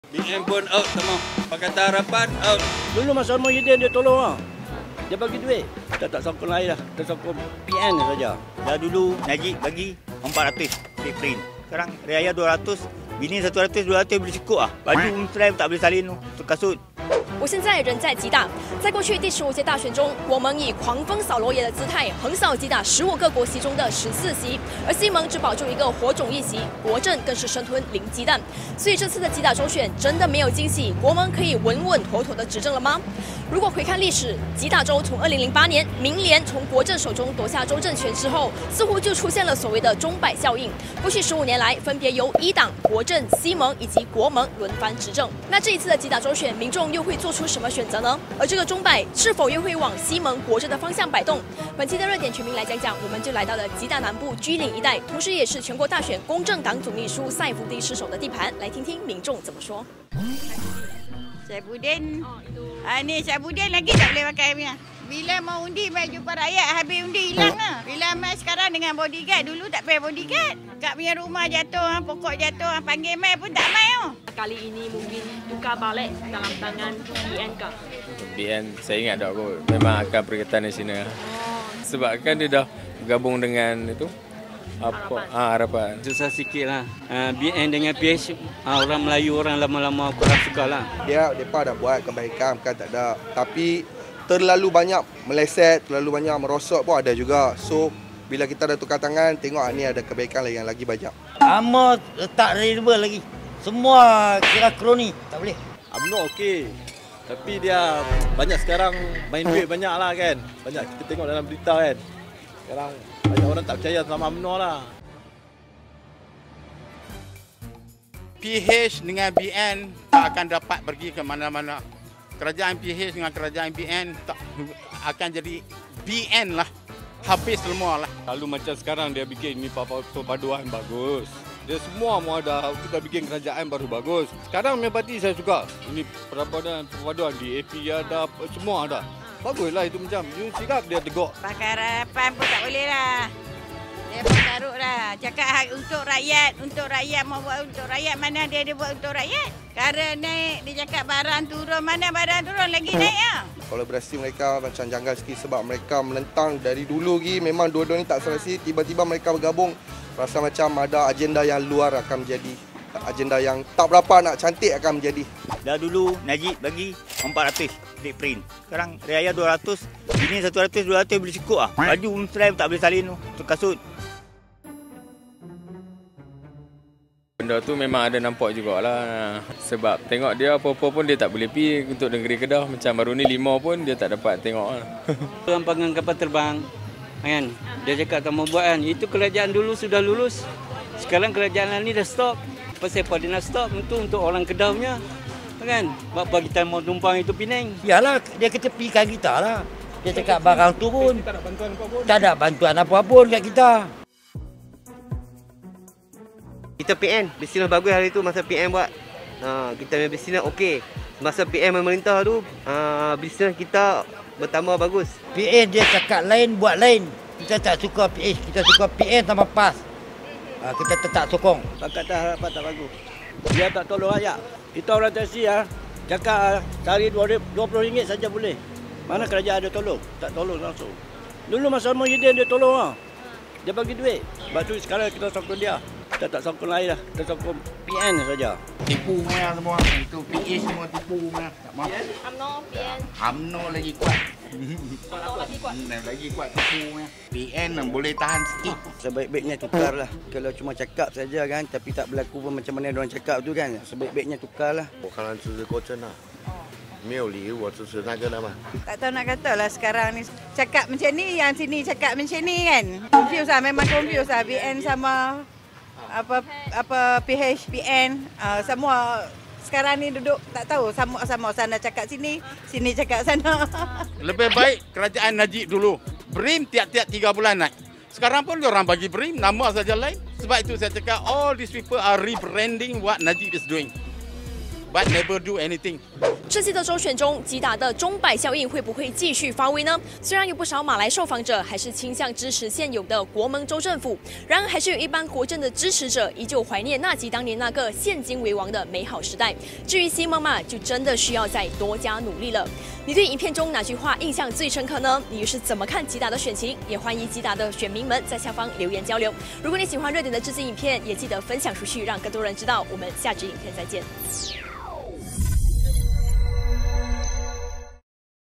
国盟或稳守吉打州政权 希盟国阵会全军覆没？
▐ 全民来讲讲▐ 吉打州过去15年来分别由民联、国阵、希盟和国盟轮番执政。这样的“钟摆效应”，这次还可能上演吗？吉打州政权又会花落谁家？

热点《全民来讲讲》团队这次北上吉打南部，听听吉打民众如何看待这次州选的选情！国盟真的稳赢？希盟国阵真的难有突破？来探听选民的“风向”吧！

热点主持｜@Celes 美伶